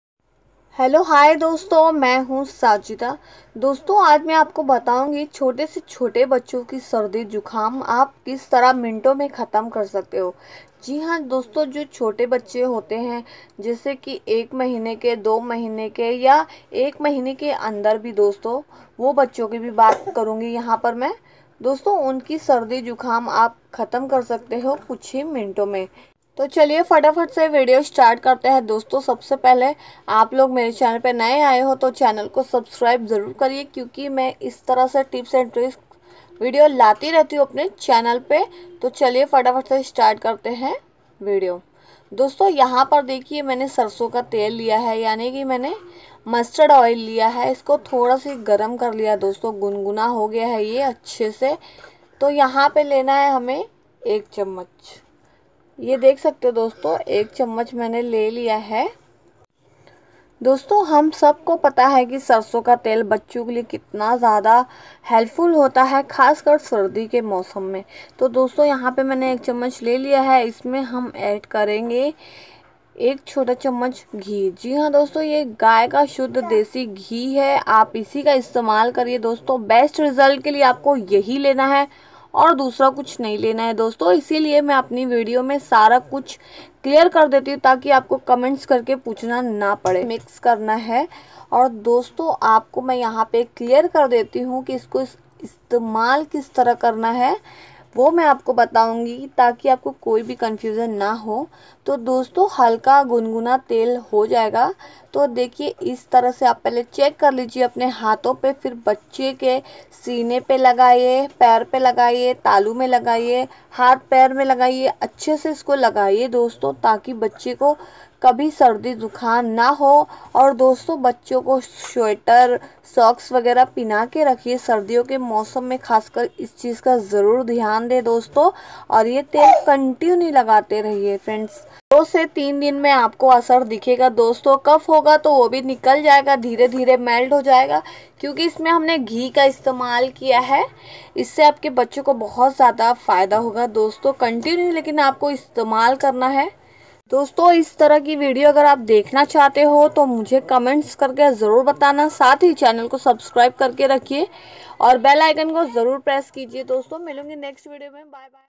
मिल गया आपके बच्चों की सर्दी जुखाम को तेजी से खत्म करें sardi jukam ke gharelu nuskhe/ sardi ka ilaj
बच्चों की सर्दी जुखाम जड़ से खत्म होगी
 जिंदगी में सर्दी कभी होगी नहीं  बच्चों की सर्दी जुखाम जड़ से खत्म होगी